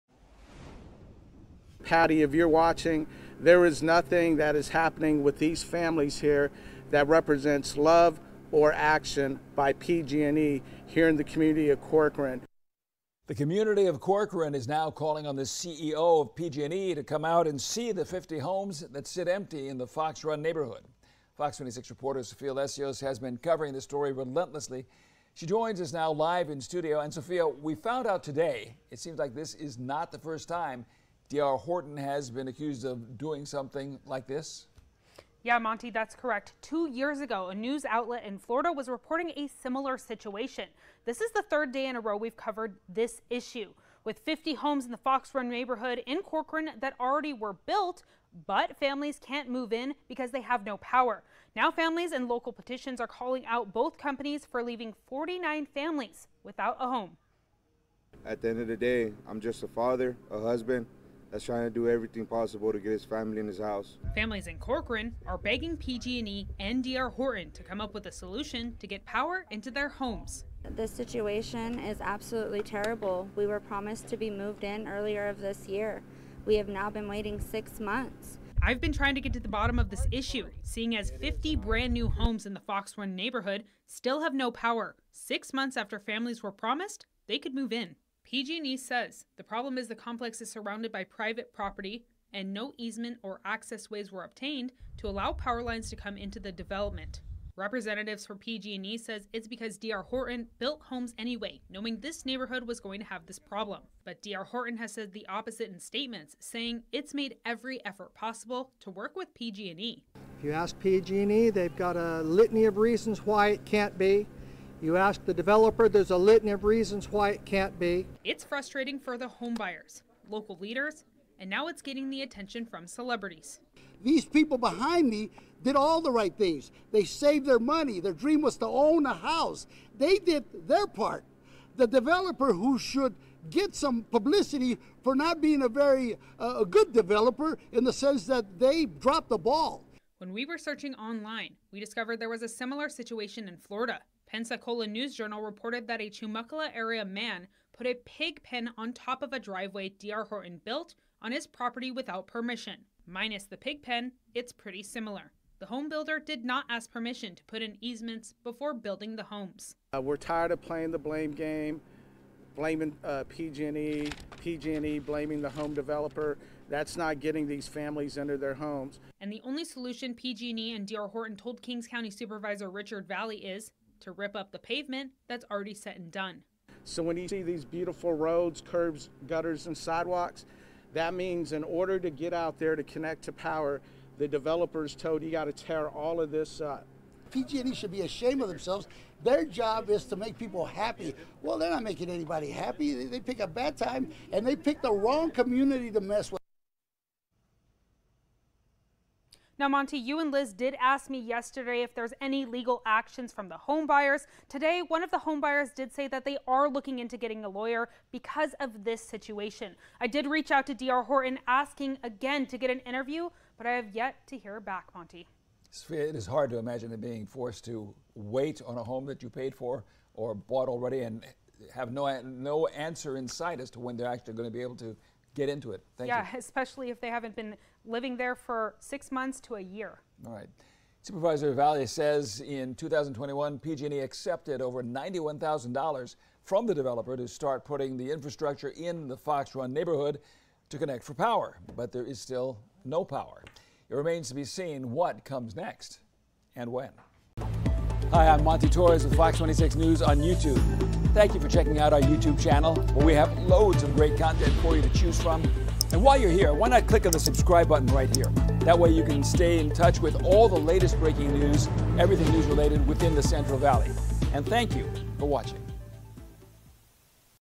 Local uproar as 50 homes remain powerless six months after move-in date
The Corcoran community is now calling on the CEO of PG&E to visit the 50 empty homes in the Fox Run neighborhood that still sit without power.